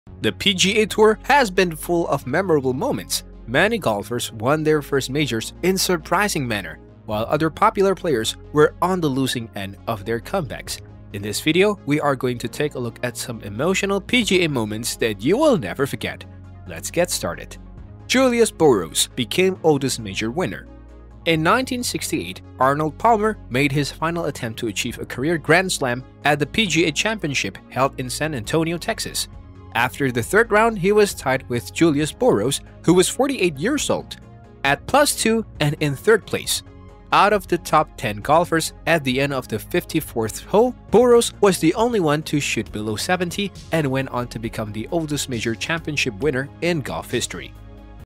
Most Emotional PGA Moments You Will Never Forget
In today's Swing Masters video we are going to be looking at Most Emotional PGA Moments You Will Never Forget